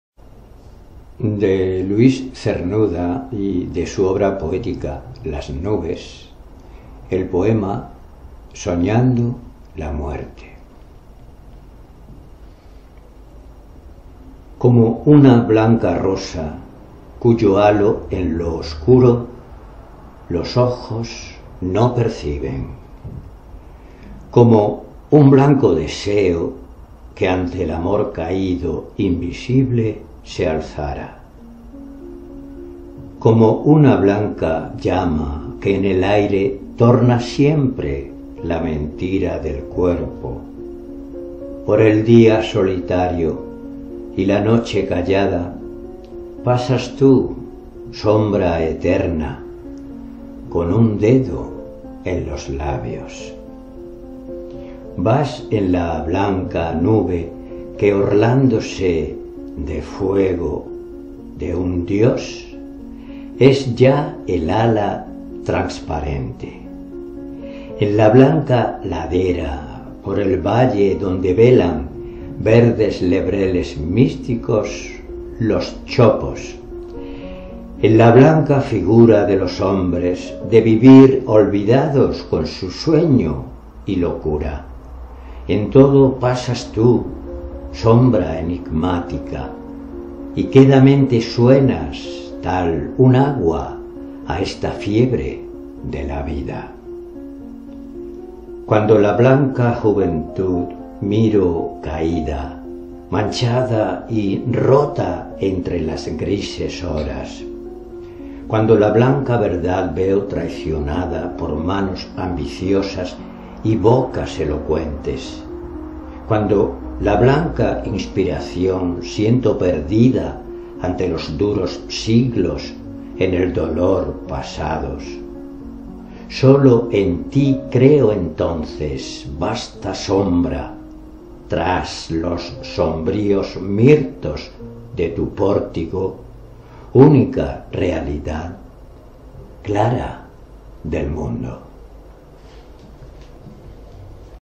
LUIS CERNUDA. SOÑANDO LA MUERTE. LAS NUBES. (1937-1940).
"Luis Cernuda Bidou o Bidón (Sevilla, 21 de septiembre de 1902-Ciudad de México, 5 de noviembre de 1963) fue un destacado poeta y crítico literario español, miembro de la Generación del 27.
El abuelo materno de Luis Cernuda, Ulises Bidou, era de origen francés, pero se castellanizó el apellido al instalarse como droguero en Sevilla; luego en el exilio su nieto recobraría el apellido.1​ Su educación fue rígida e intransigente debido al carácter y a la condición militar de su padre, Bernardo Cernuda y Bousa, que se retiró con el grado de coronel de ingenieros. Con motivo del traslado de los restos de Bécquer, a los nueve años de edad empieza a leer poesía y, más tarde, un profesor lo anima a escribir versos y le corrige los que compone."(Wikipedia).